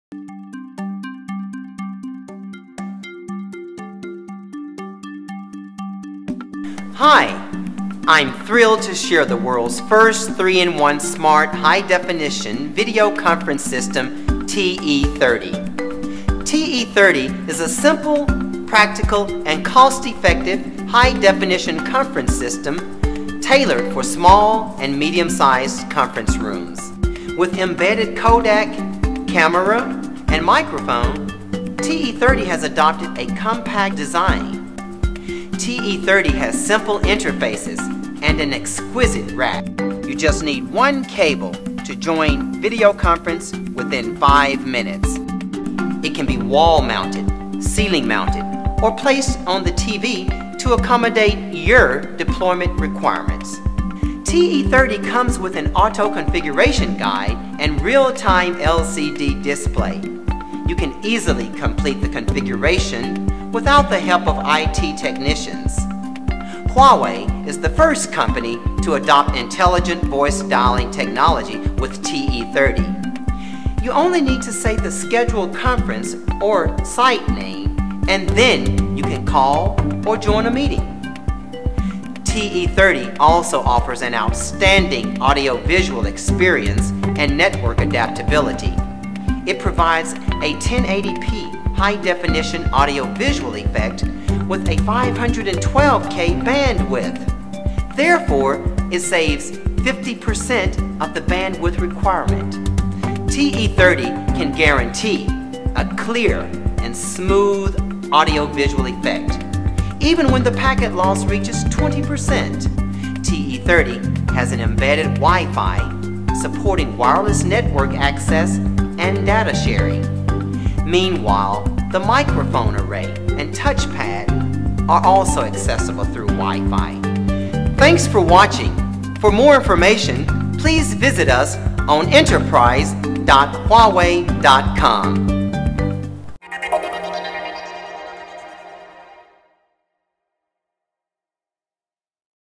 Huawei VPC600/VPC620 HD Video Camera
Videoconferencing systems are increasingly moving toward high-definition (HD) video and high-fidelity (hi-fi) audio. Driven by customer demand, Huawei has developed HUAWEI VPC600/VPC620 HD PTZ video camera. Used with an ultimate series of Huawei's videoconferencing endpoints, it delivers a complete set of 1080p 60fps HD videoconferencing services, bringing clear, crisp and natural images. By supporting the 3G-SDI standard, the VPC620 HD PTZ can be placed up to 60 feet away from its target conferencing system, delivering maximum deployment flexibility.